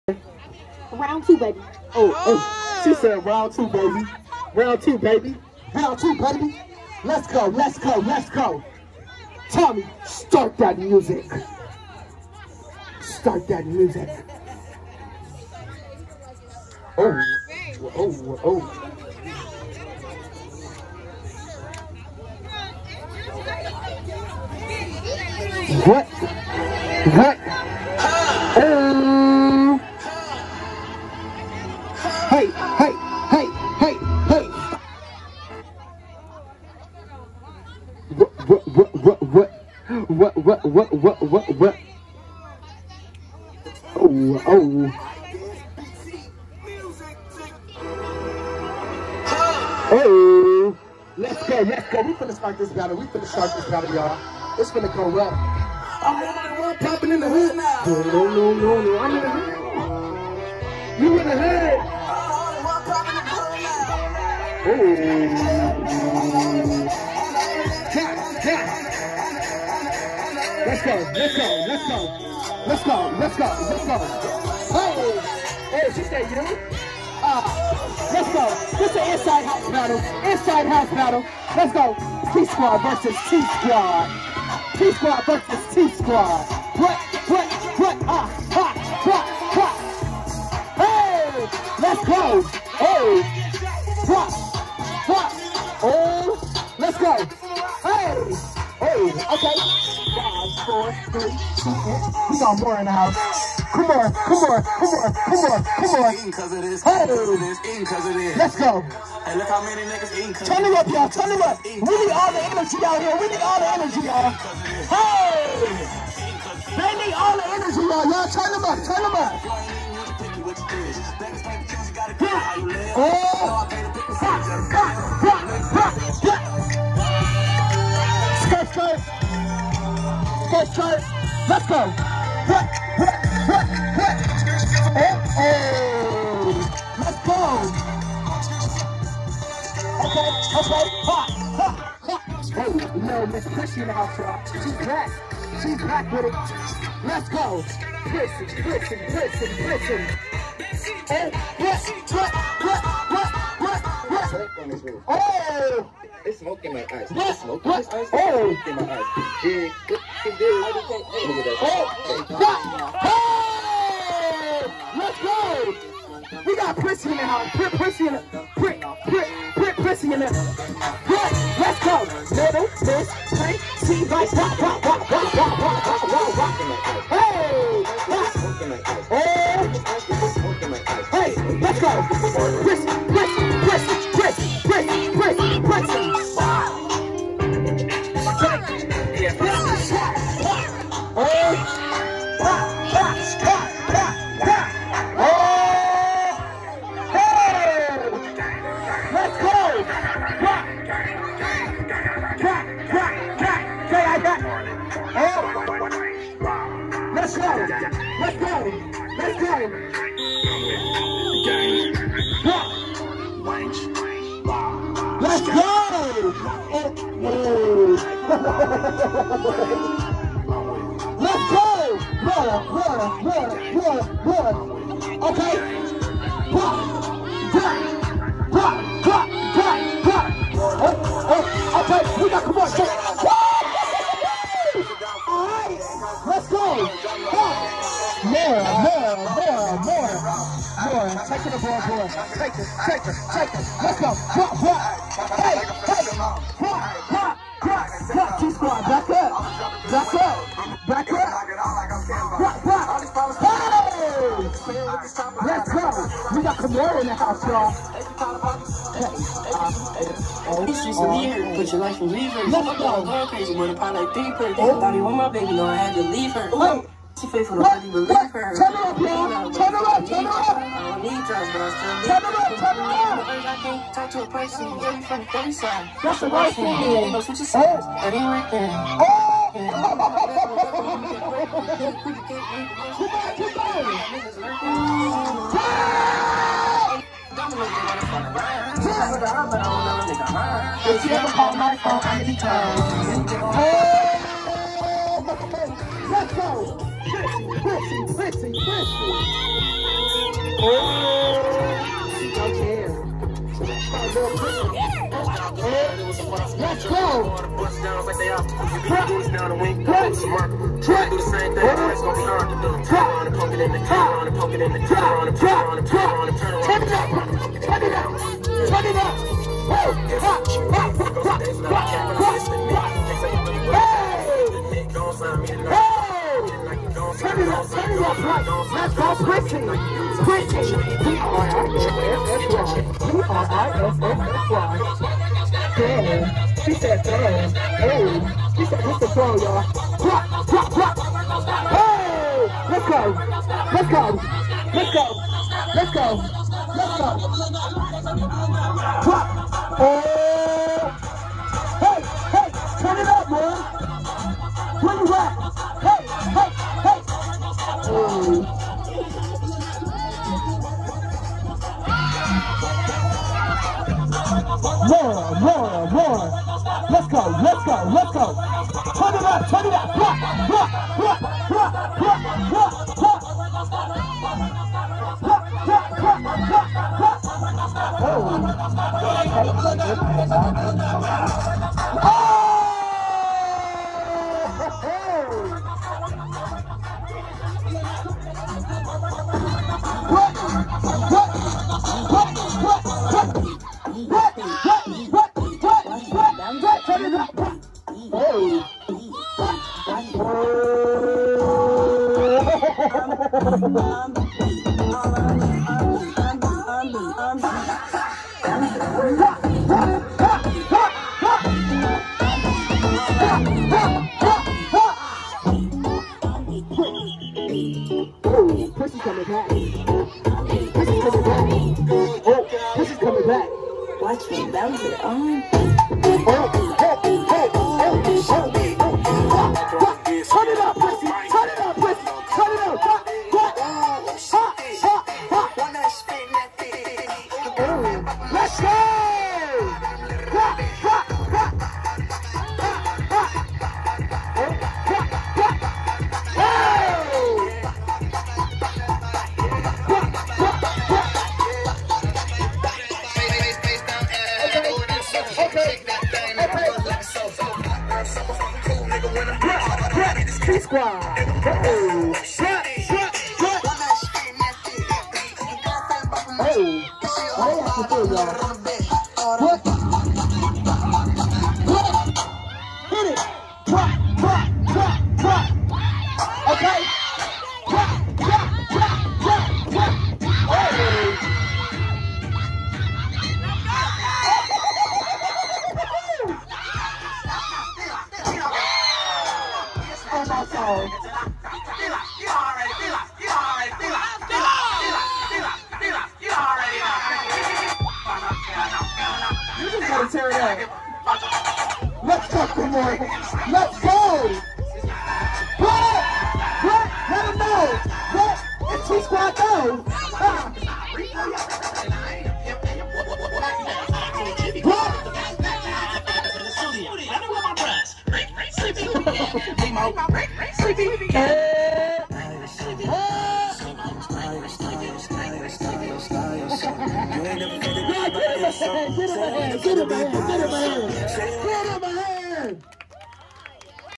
WHAT CAME OUT OF HER * YOU BE THE JUDGE * FOLLOW @TOMMYTHECLOWN ​
Tap the * to follow and get notified!

KIMORA VS LIL MISS PRISSY  THEY GOT OFF THE BATTLE IS HOT LIKE FIRE  YOU BE THE JUDGE